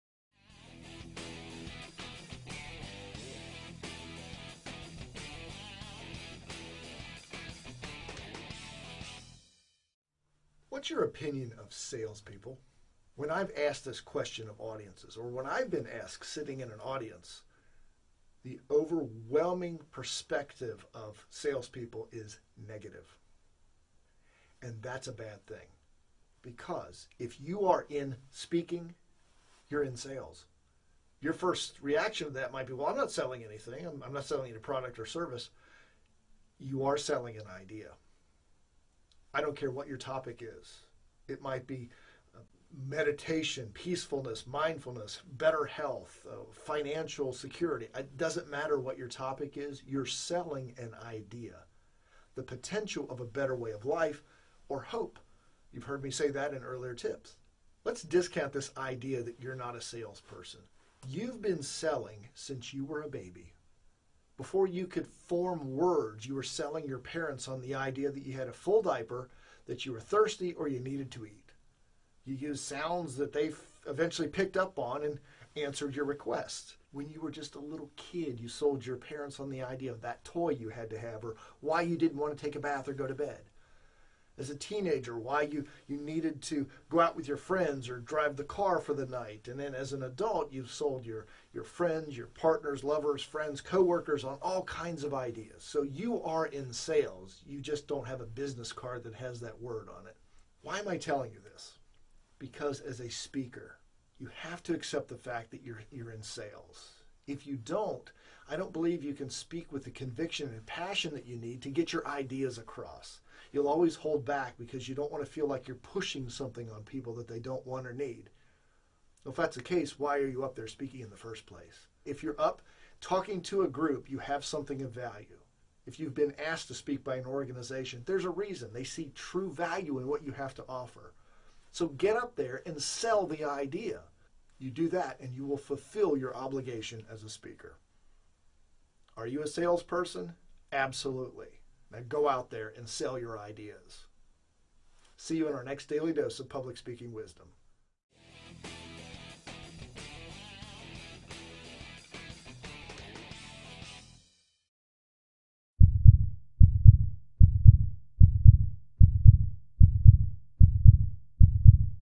Are You Willing to Accept This Hard Truth About Speaking?
The words 'salesman" or "salesperson" are often looked upon as negative.

If this is how you view them, consider reevaluating your perception. This video tells you why.

Do you have a specific question about public speaking you'd like Michael to answer? If so, type it in the comments box.